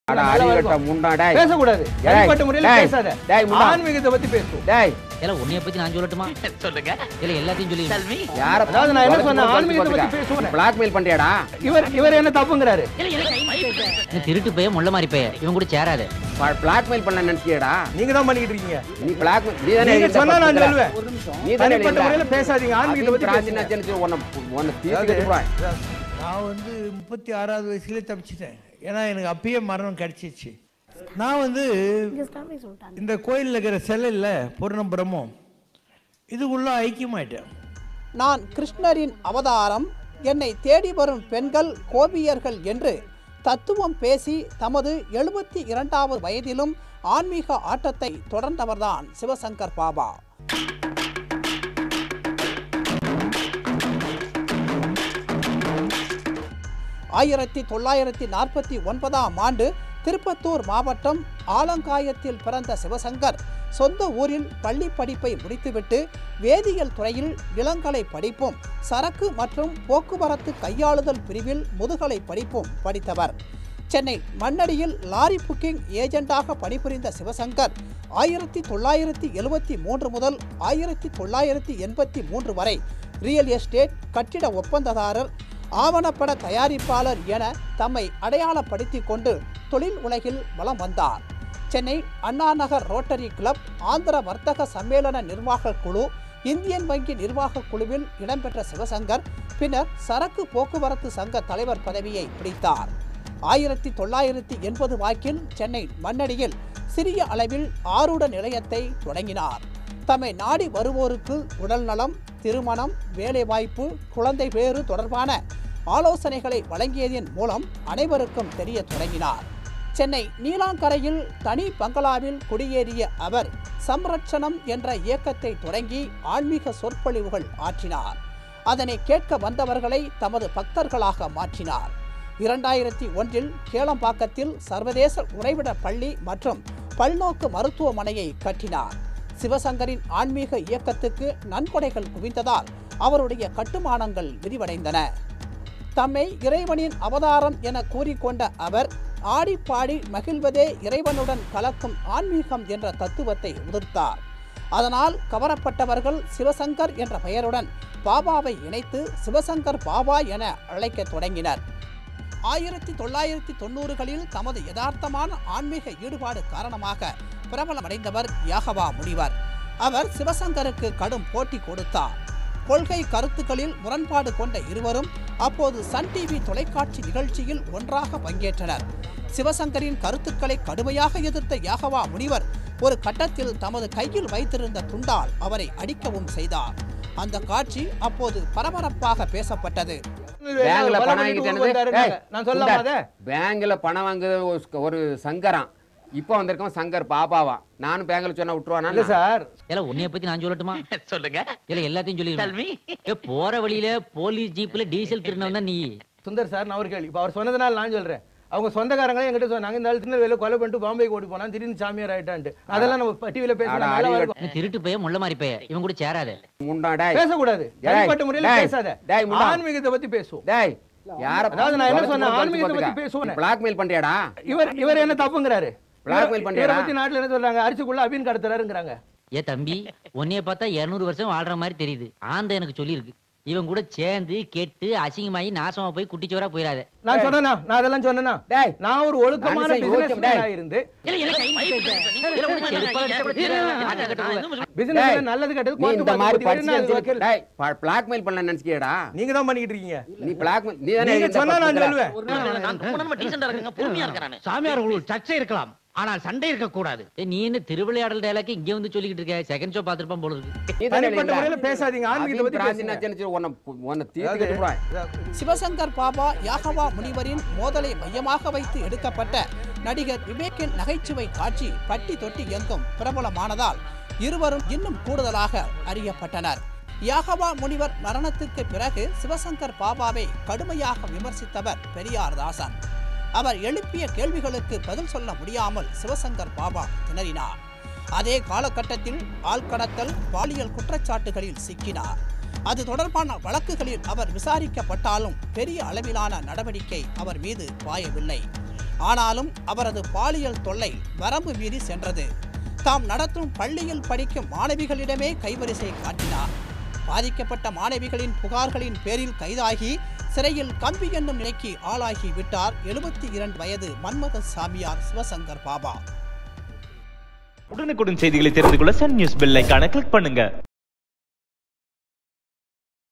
சிவசங்கர் பாபா Vs யாகவா முனிவர் பிரபல விவேக் - மயில்சாமி காமெடியின் பின்னணி | Sivashankar baba
SivashankarBaba | VivekMayilsamyInterview | VivekMayilsamySunNews | விவேக் காமெடி பாணியில் பாபாவுடன் முனிவர் மோதல் | Sivashankar baba Vs Yagava Munivar

 Sivashankar baba Vs Yagava Munivar | Vivek Mayilsamy Sun news